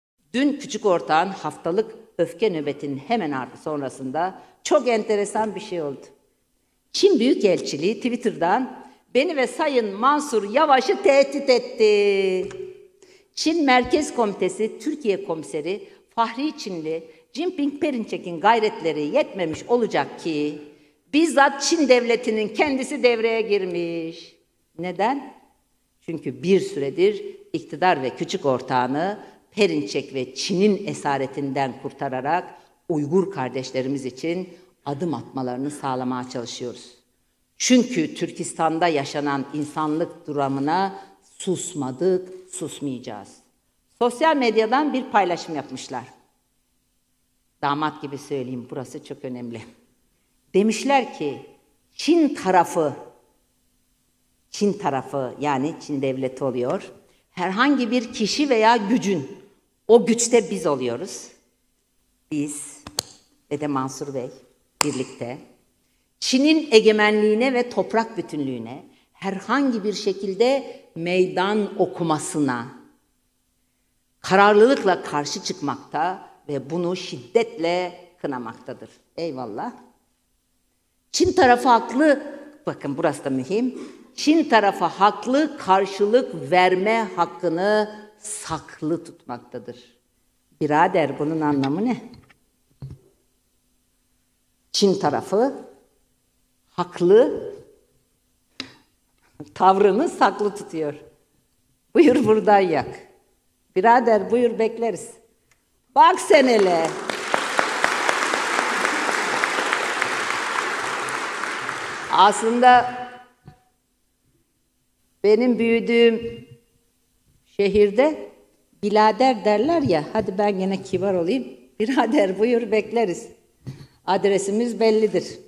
"Çin Büyükelçiliği Twitter'dan beni ve Sayın Mansur Yavaş'ı tehdit etti." Saray buna seyirci kaldı
Dün küçük ortağın haftalık öfke nöbetinin hemen sonrasında; Çin Büyükelçiliği Twitter'dan beni ve Sayın Mansur Yavaş'ı tehdit etti. Fahri Çinli Cinping Perinçek'in gayretleri yetmemiş... Çın tarafı haklı tavrını saklı tutuyormuş. Birader buyur bekleriz... Adresimiz bellidir.